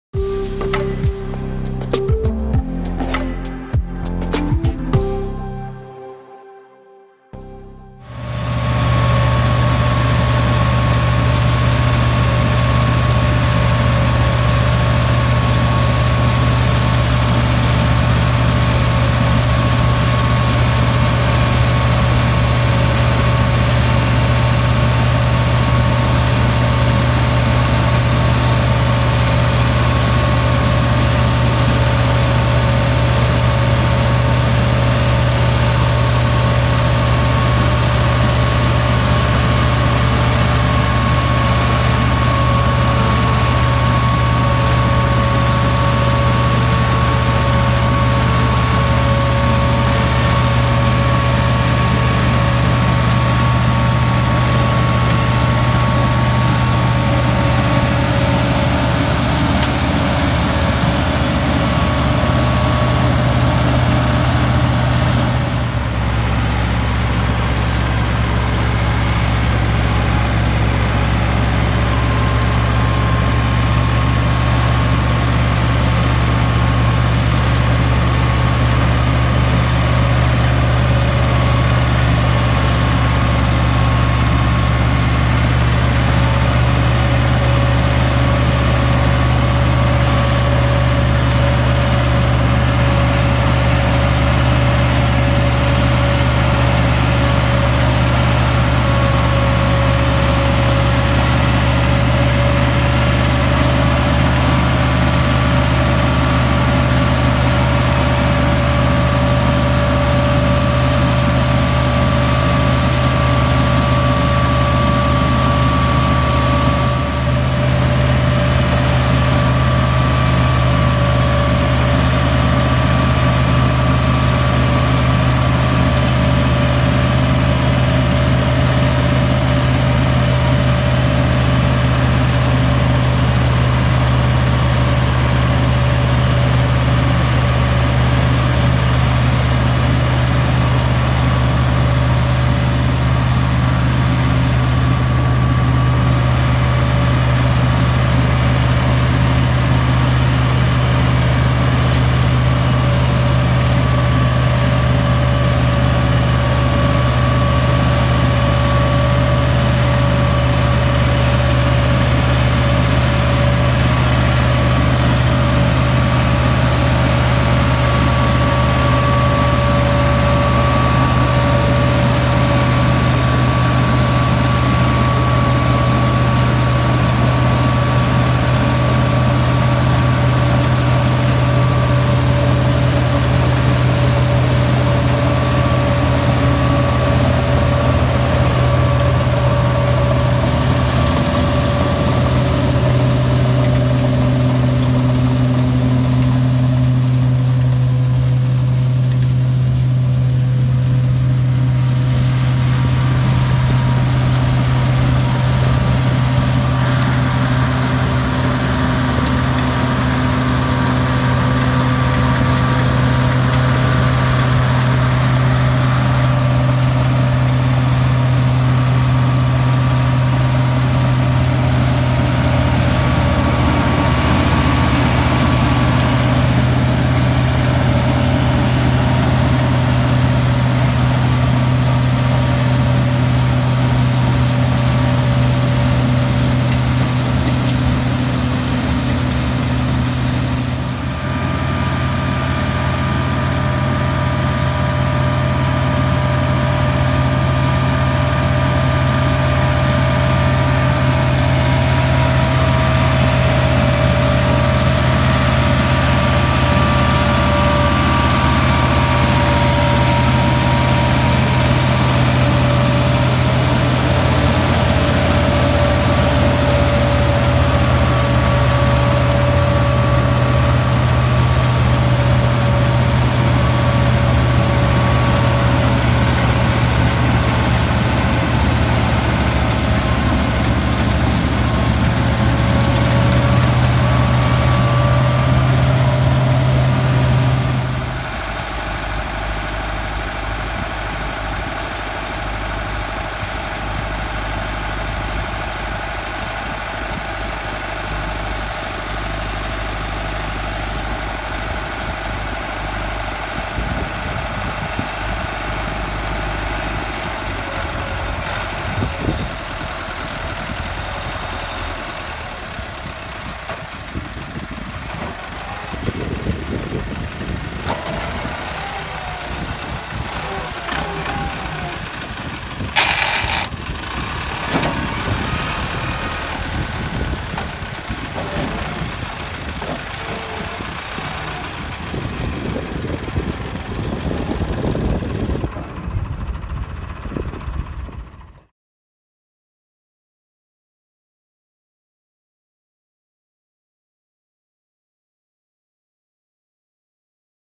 Massey Ferguson 385 | Umar Wheat Straw Chopper | PTO Power of Massey Tractor
In this video Massey Ferguson 385 operating wheat straw chopper machine with PTO shaft. Amazing performance of tractor and machine. Massey Ferguson 385 is 85HP 4 cylinder engine. Massey tractors are best for trolley & Trailer and other agricultural appliances.
If you like my video Plz click the Like button and share.

Tariq Naveed